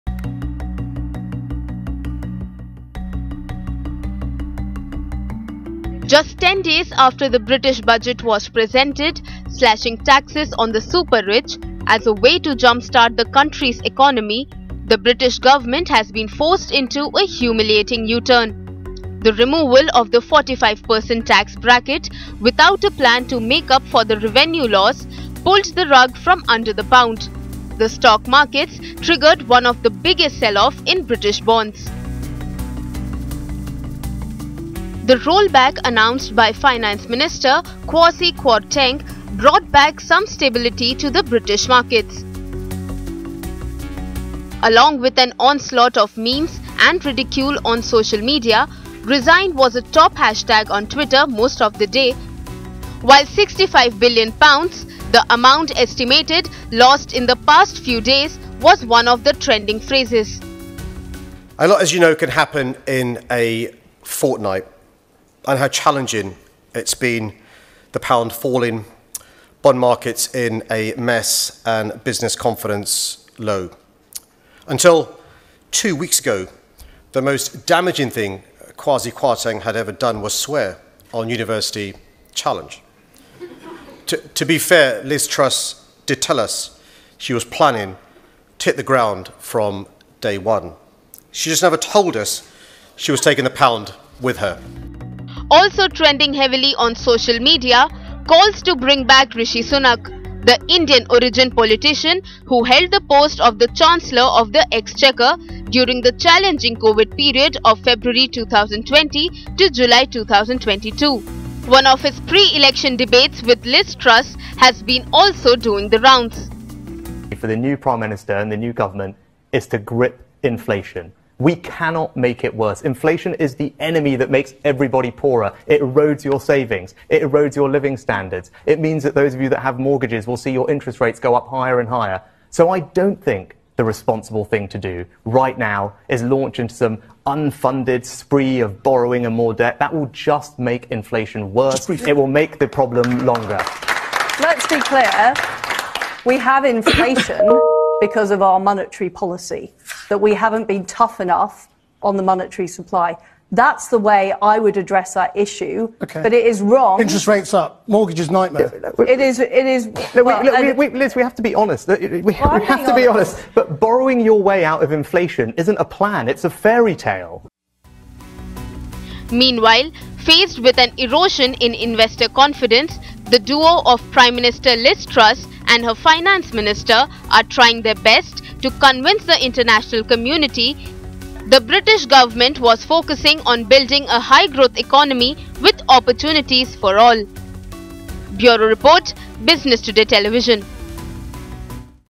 Are The Brits Missing Their Former Chancellor Of The Exchequer, Rishi Sunak?
It has not been a month yet, and British Prime Minister Liz Truss’s government is facing massive humiliation with a U-turn on a tax cut that led to disastrous consequences for the Pound and forced the Bank of England to spend 65 billion pounds to stabilise government bonds. This has spawned a new rallying cry across the UK, bring back Rishi Sunak.